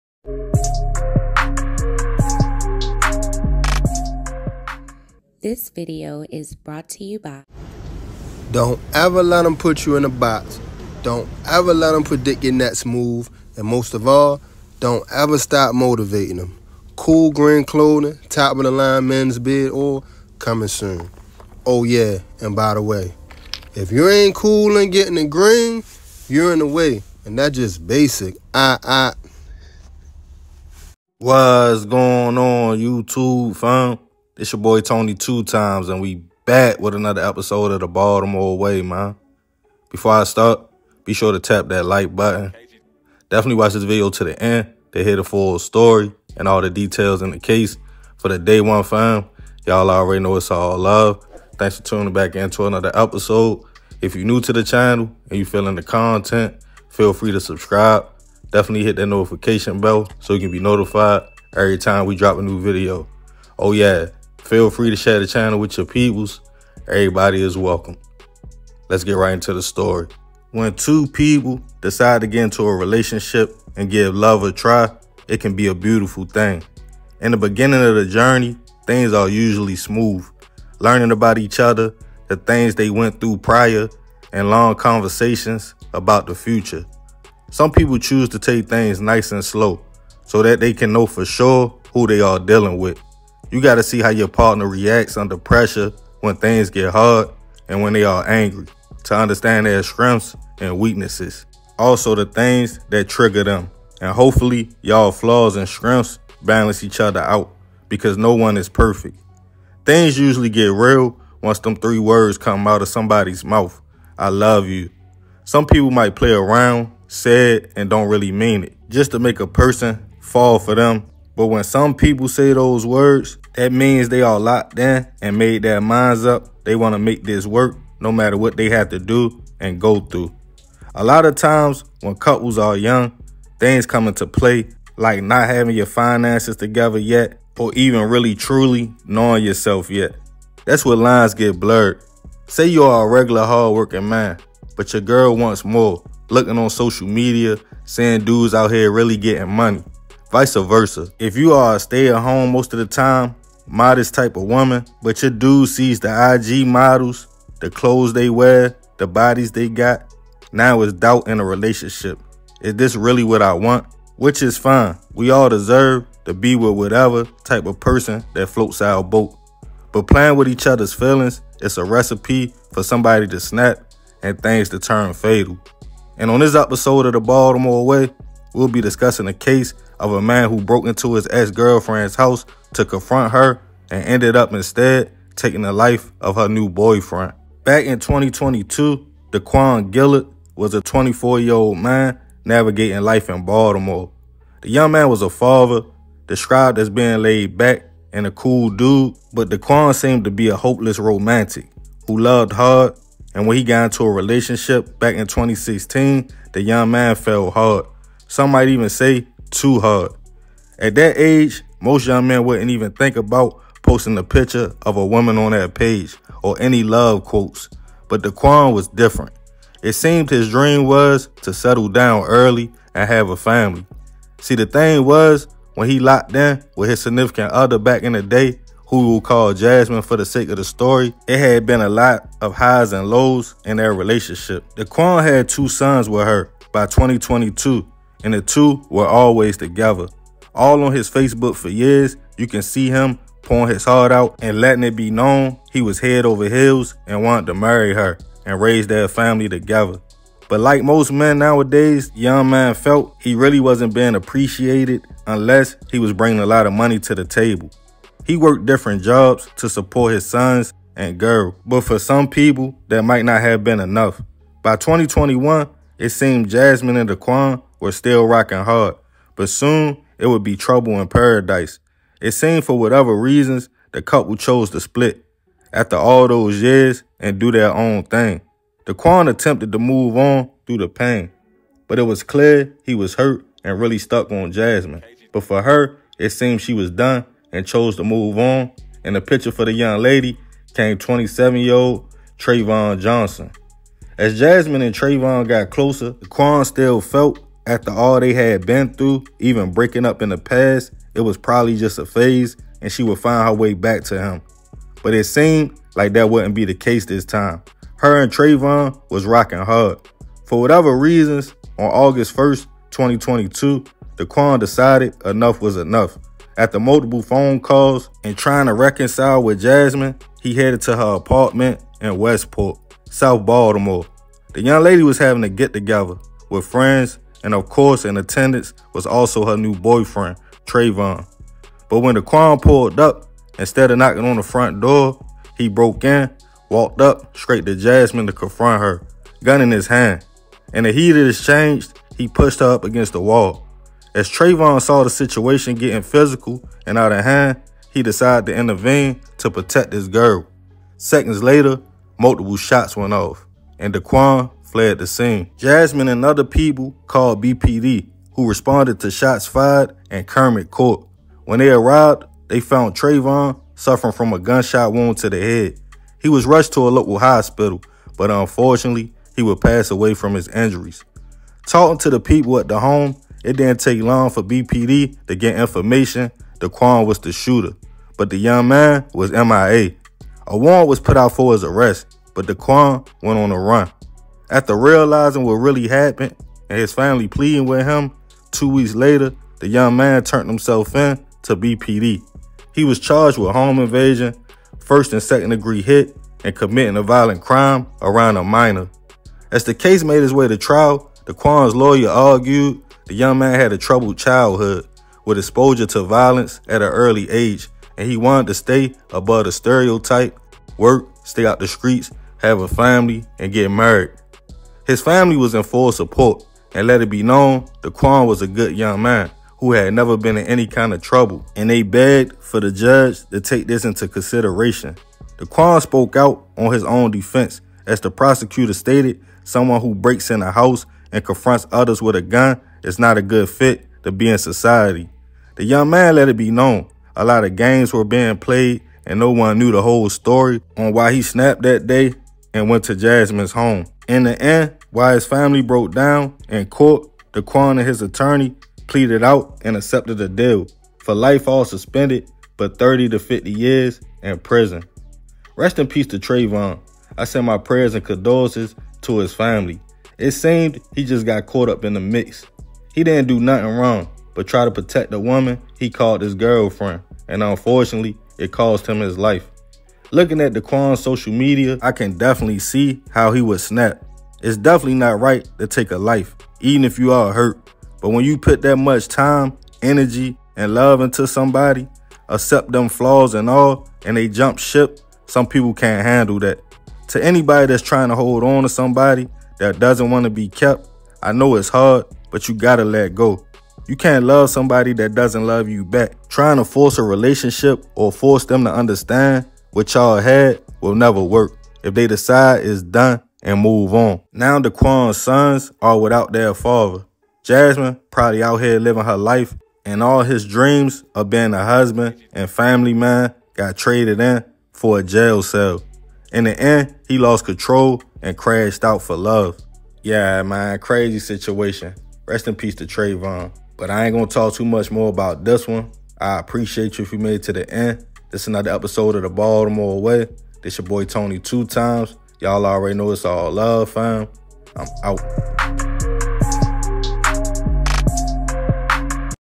He Went To Confront His Ex-Girlfriend But Ended Up Taking Her New Man’s Life
On this episode of The Baltimore Way we will be discussing a case from Westport Homes in South Baltimore of a man who went to confront his Ex-girlfriend but ended up taking her new man’s life.